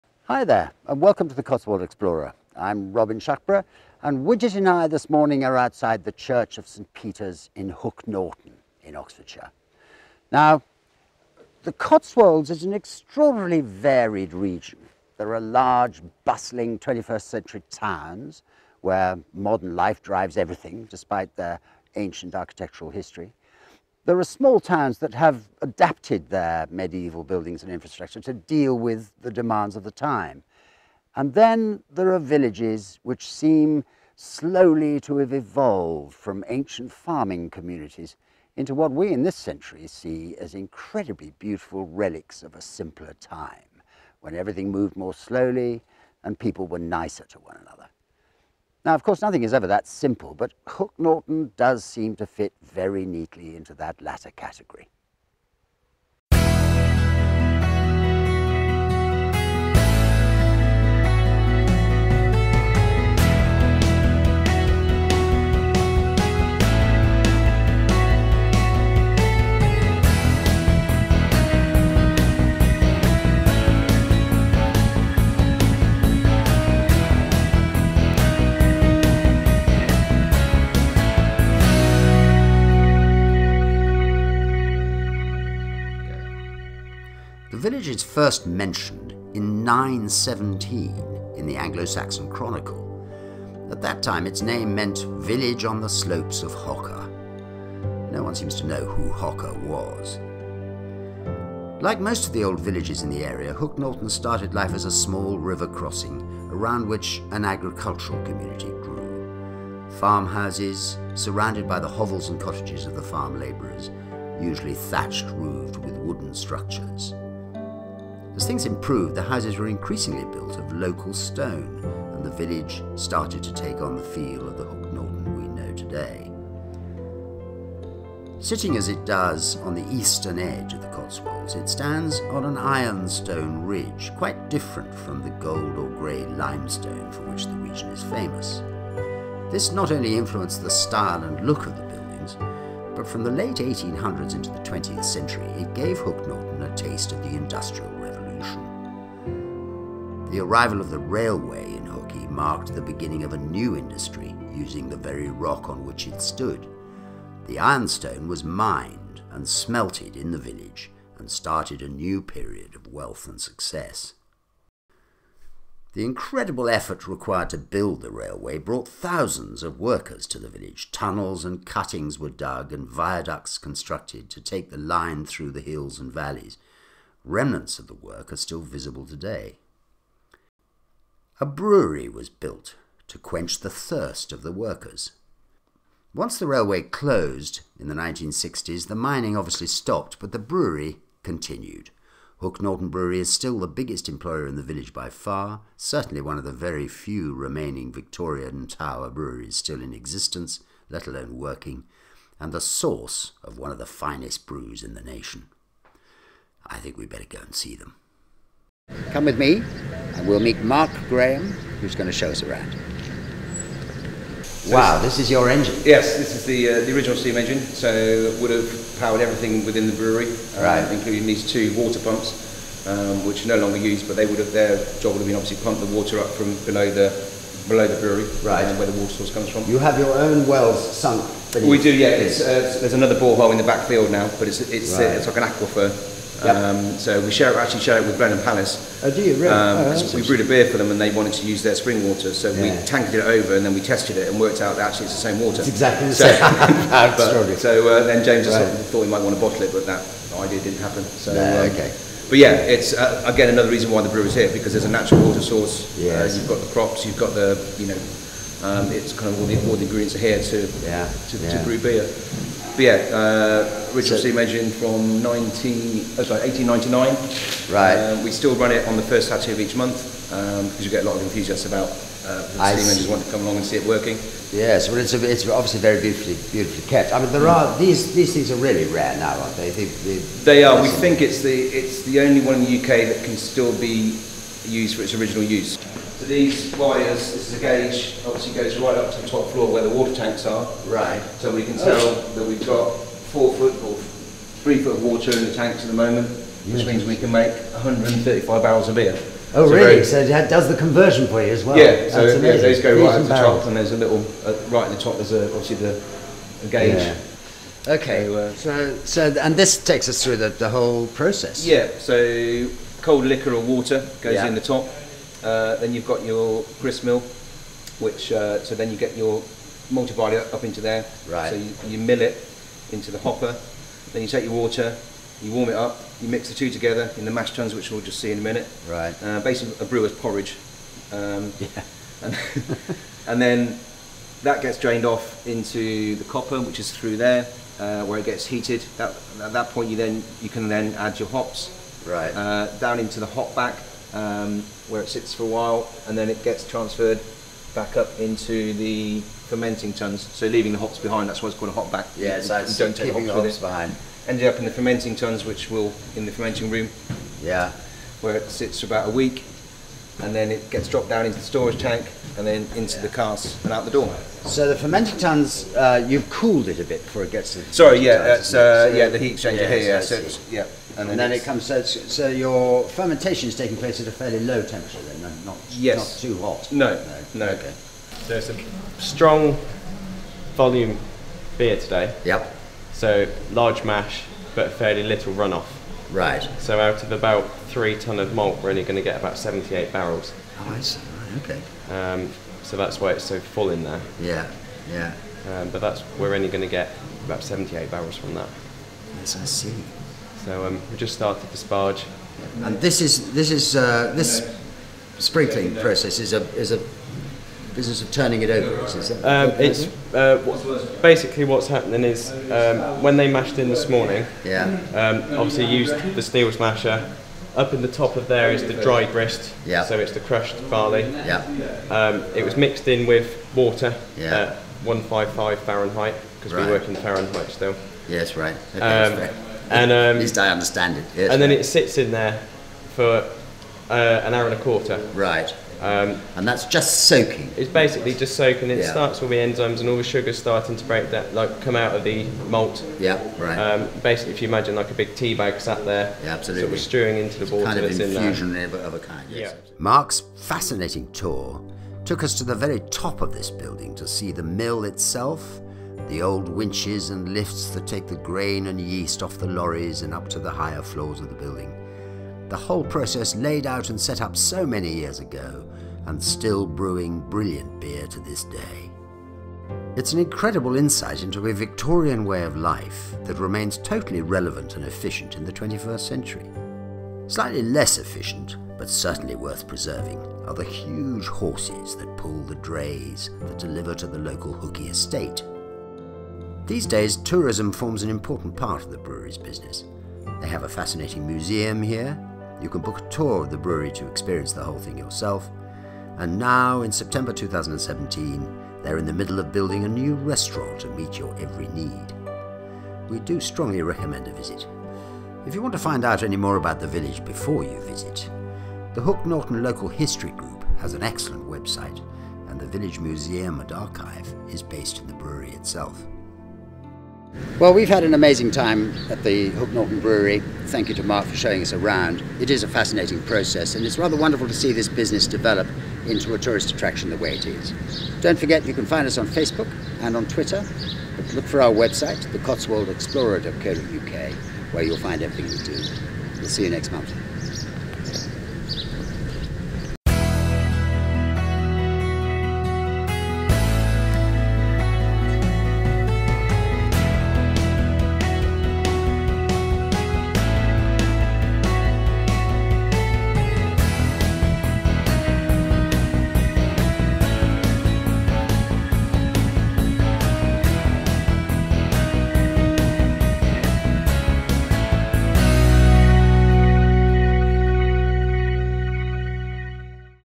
A History of Hook Norton and the Brewery | Exploring the Cotswolds
In this months exploratory trip around the Cotswolds, we visit Hook Norton, and see the inner workings of the Hook Norton Brewery.